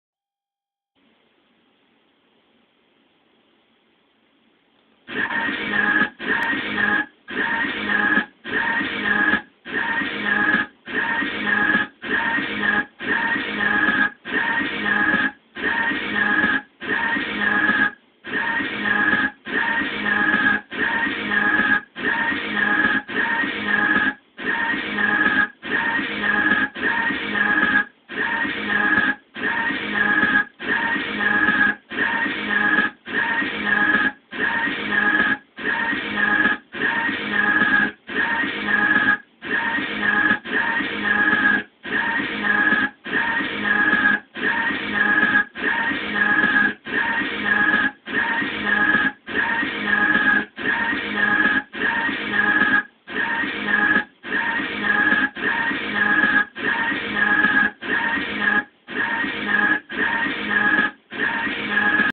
Video Repetition No. 2 (Nirvana)
An avant-garde experiment using repetition and inverted colors.